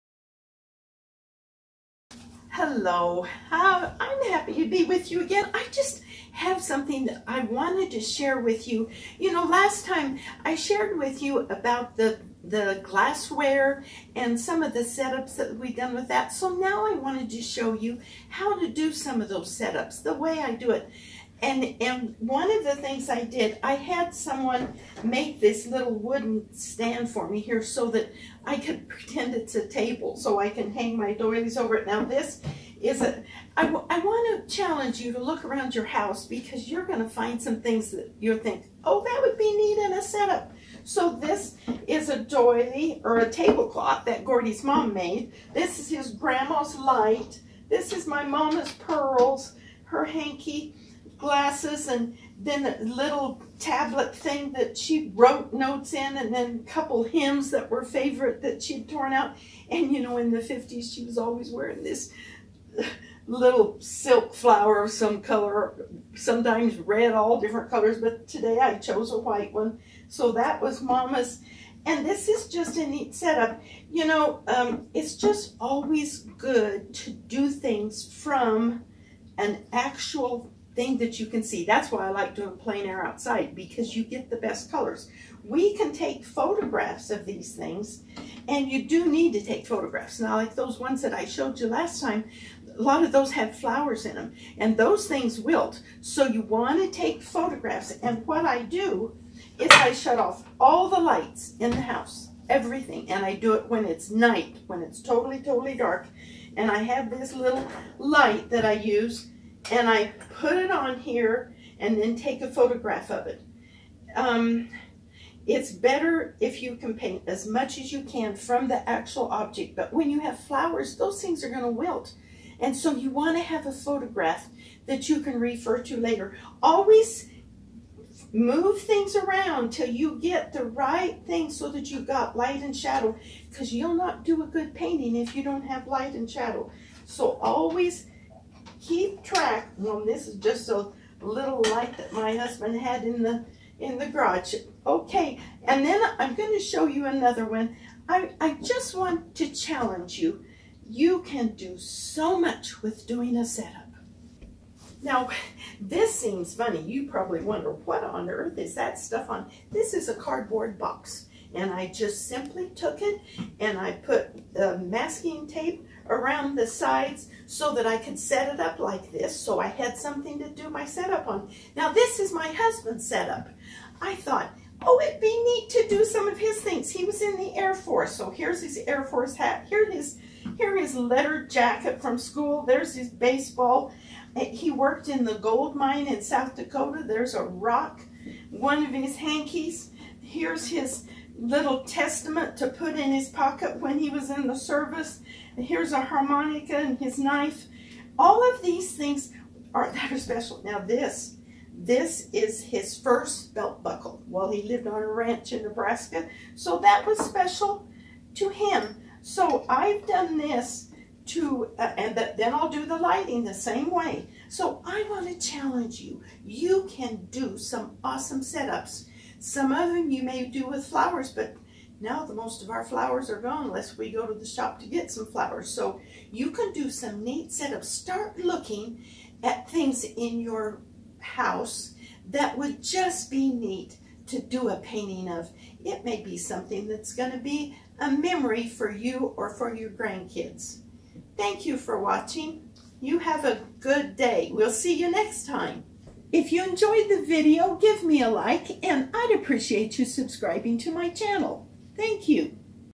How To Set Up Still Life For Painting
This week, I'm challenging you to find things around your house to set up for still life painting. In the last video, we talked about painting glassware, and in many of those you can see my still life paintings of flowers. Flowers are nice, but this time of year we have to find other items to paint. So, I challenge you to find something different right now. Perhaps you have antique tablecloths or heirloom jewelry around your house, those are an inspiration to paint. Turn off all the lights, turn on your photo lighting, arrange your set up, take a snapshot and paint what you've got! Thank you for tuning in today. I hope you have enjoyed this video.